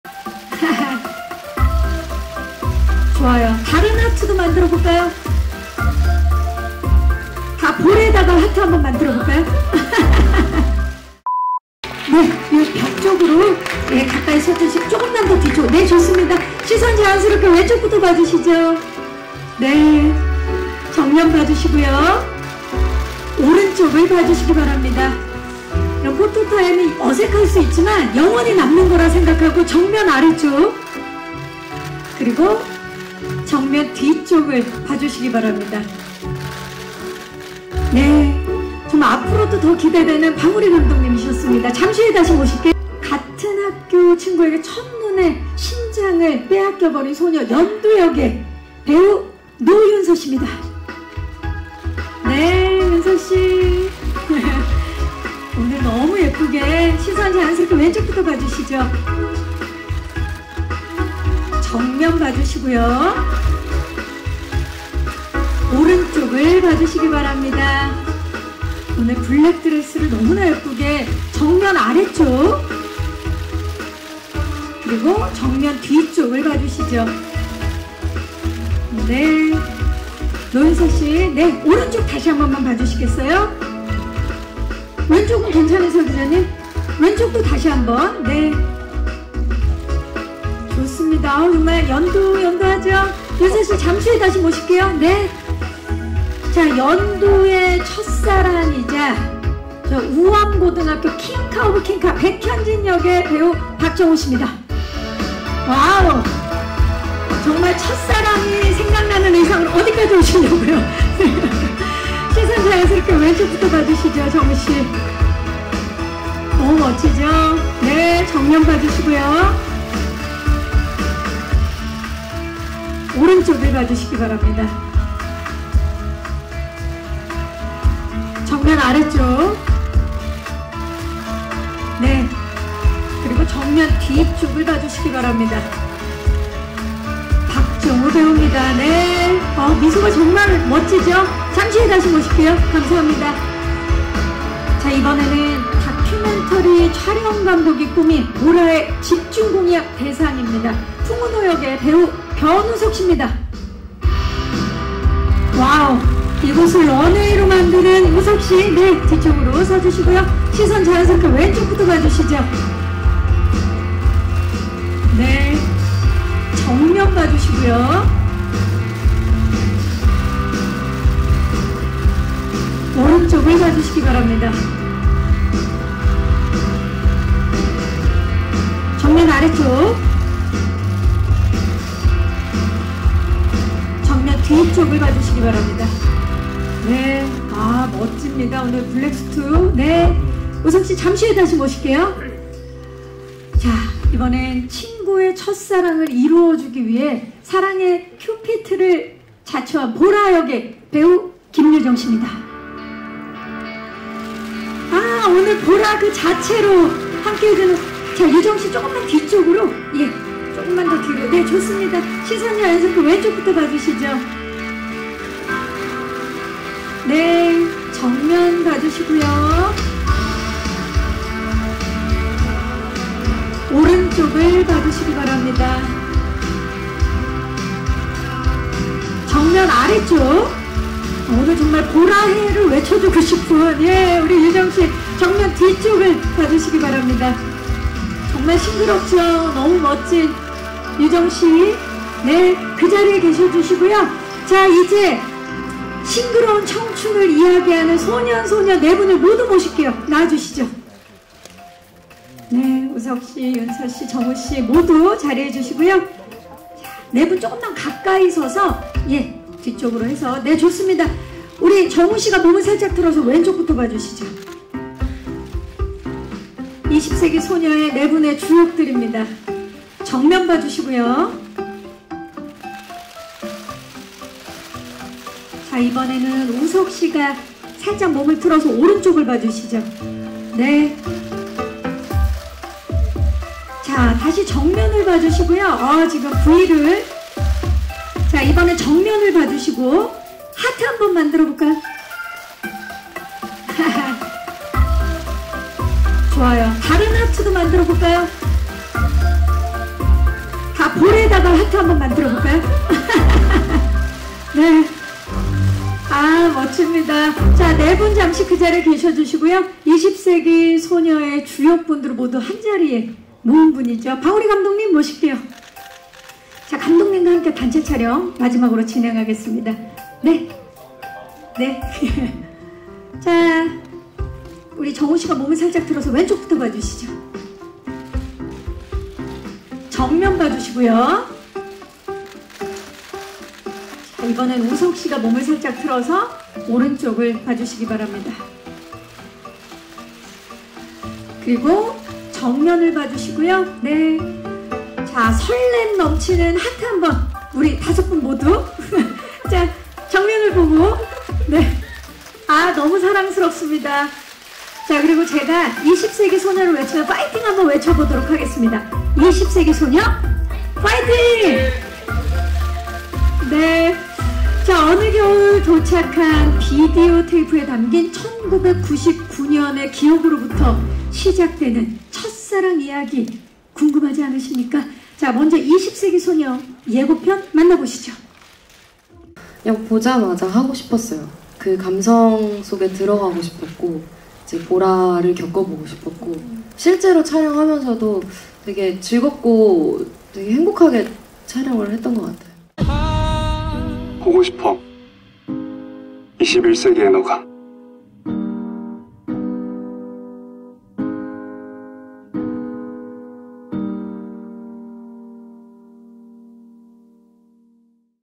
[photo time] 김유정(Kim You Jung) X 변우석 X 박정우 X 노윤서(Roh Yoon Seo)ㅣ넷플릭스 '20세기 소녀' 제작보고회
청춘을 대변하는배우 김유정, 변우석, 박정우, 노윤서의 만남으로 화제를 모은 넷플릭스(Netflix) 영화 '20세기 소녀'이 10월 19일(수) 오전 서울 용산 CGV용산아이파크몰에서 제작보고회를 개최했다.

MC 박경림의 사회로 진행된 이날 제작보고회에는 김유정, 변우석, 박정우, 노윤서, 방우리 감독 등이 참석했다.